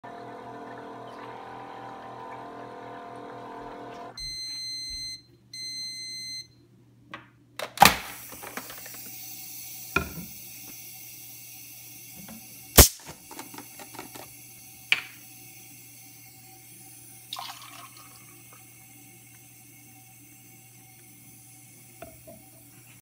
Pumpkin Pie Cocktail - End of Sparkle Cycle and Pour
Celebrate the changing season and practice your Halloween mixology with this Pumpkin Pie Cocktail. This cocktail is full of rich, real pumpkin taste, complementary spices and finished with fine bubbles to enhance all the flavors.  See how to create it at: